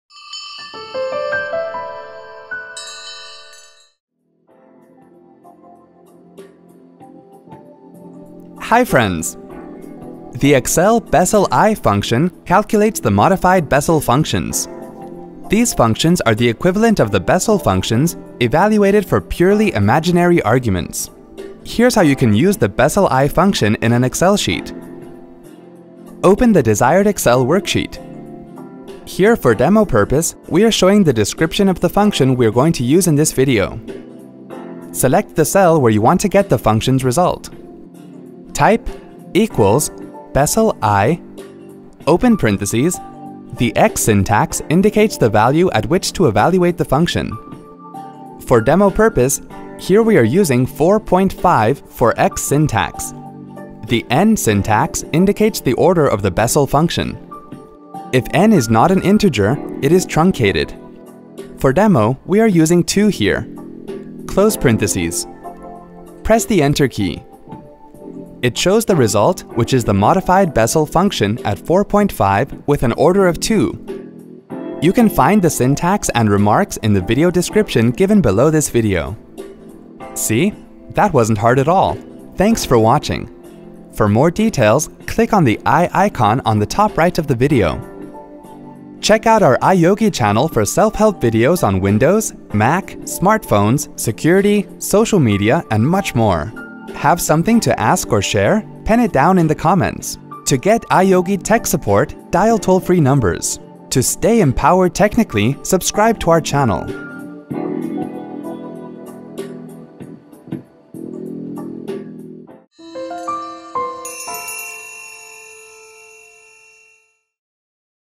How to use the BESSELI function in Excel :Tutorial
The Besseli function in Microsoft® Excel calculates the modified Bessel functions. These functions are the equivalent of the Bessel functions, evaluated for purely imaginary arguments. Watch this video to use the BESSELI function in Microsoft® Excel.

The syntax of the Besseli function is :
BESSELI( x, n )
where the function arguments are:

x - The value at which the function is to be evaluated
n - A positive integer, representing the order of the function

(If the supplied value of n is a decimal, this is truncated to an integer by Excel)